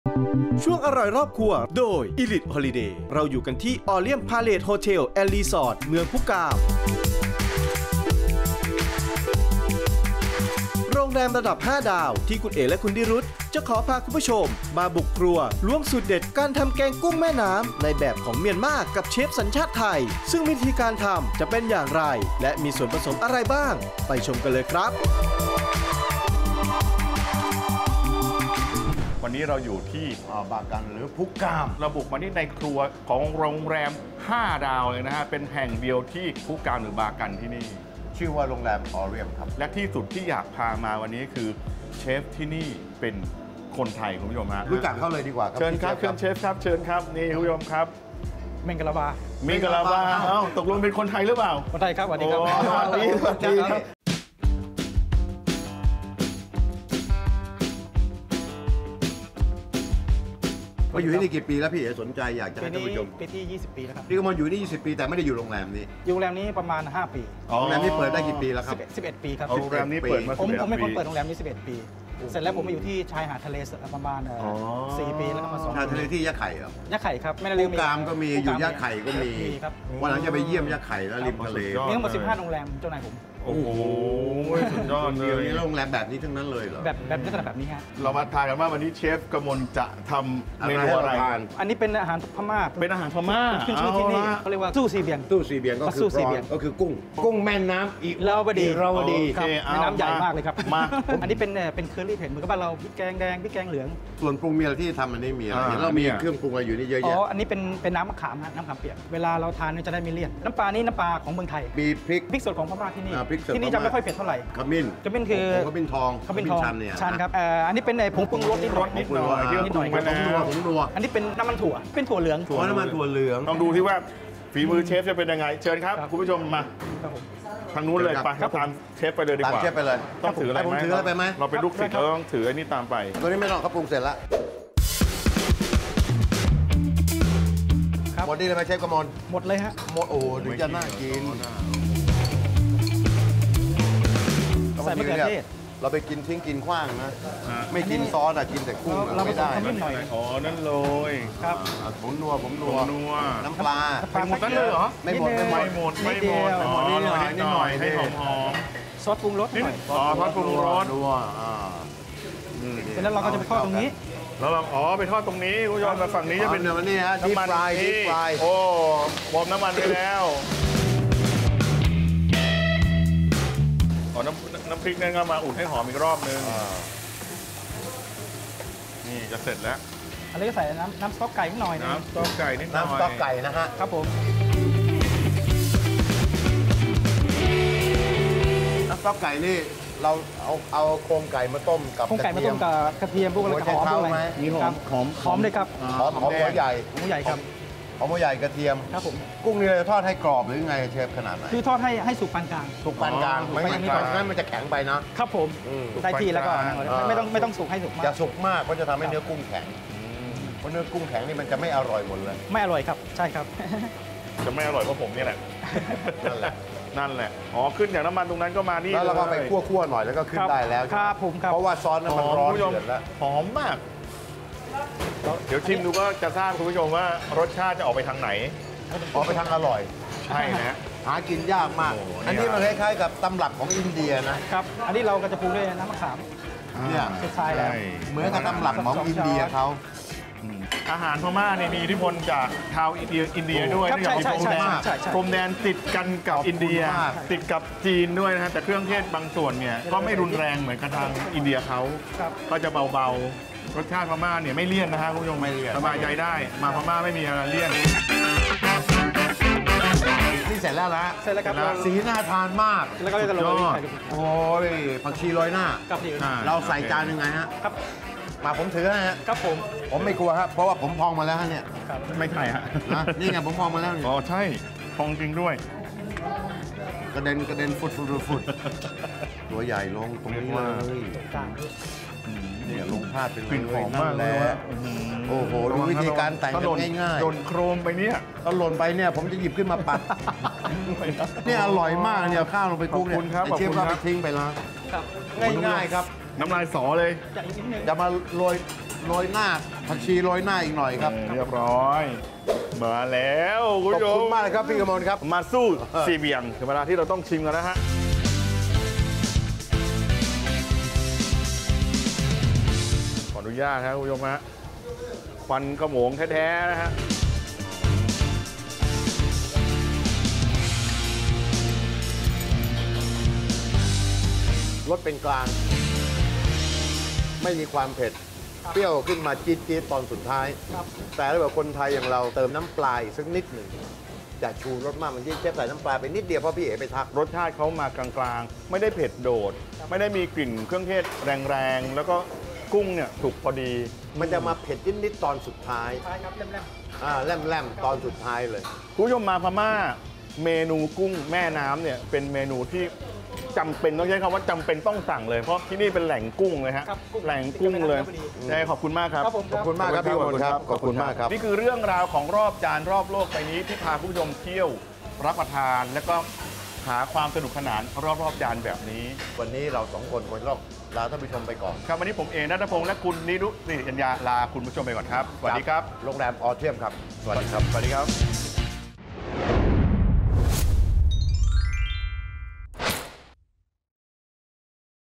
ล้วงสูตรลับ กุ้งแม่นํ้าจากประเทศเมียนมา I รอบจานรอบโลก
วันนี้อิลิท ฮอลิเดย์ ขอพาทุกคนมาล้วงสูตรลับวิธีการทำอาหารของประเทศเมียนมานั่นคือ "ตู้สี่เลียน" หรือ "กุ้งแม่นํ้าอิรวดี" นั้นมาจากแม่นํ้าอิรวดีซึ่งเป็นแม่นํ้าอีกหนึ่งสายที่สวยที่สุดในโลก แม่นํ้าสายนี้ไหลมากเหนือจรดใต้ผ่าน ประเทศเมียนมาซึ่งเป็นแม่นํ้าที่ใหญ่ที่สุดในประเทศและเป็นเส้นทางการค้าอีกด้วย